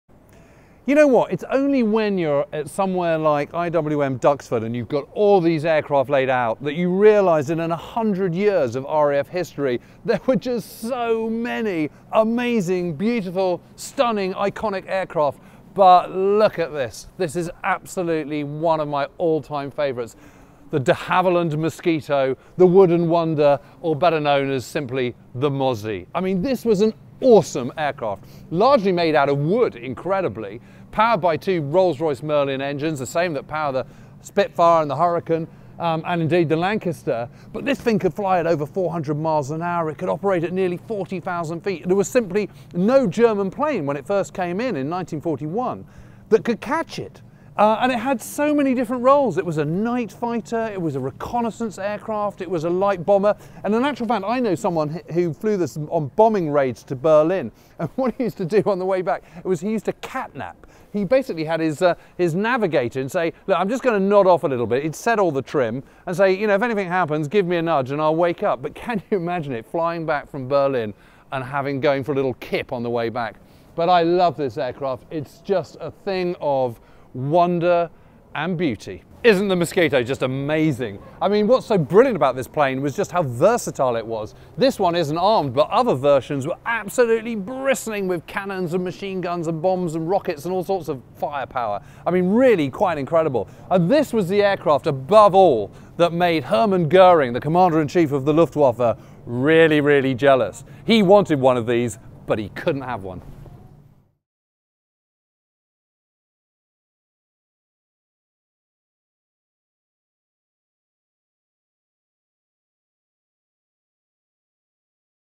James Holland Presents the de Havilland Mosquito | RAF 100 Mini-Series at IWM Duxford
In part seven of our RAF 100 mini-series, James Holland talks about the de Havilland Mosquito at Duxford IWM. His latest book "RAF 100: The Official Story" is out now.

& all good bookshops